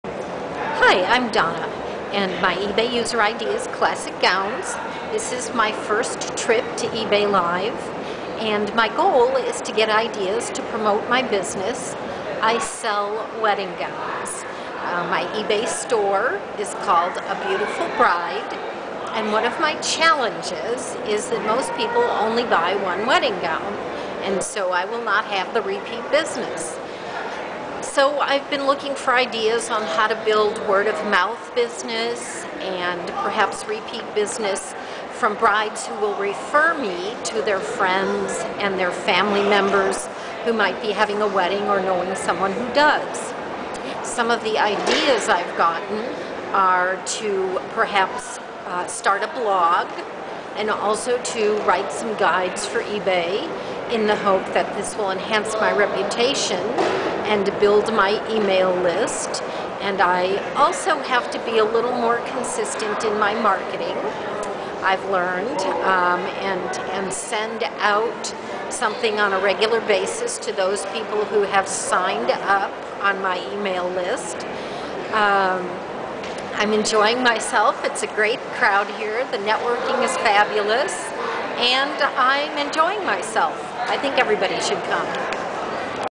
First time eBay Live! Visitor has a unique challenge
Donna from Washington sells wedding gowns. A niche where repeat business is scarce. Donna speaks about some of her ideas to get new business vs. repeat business.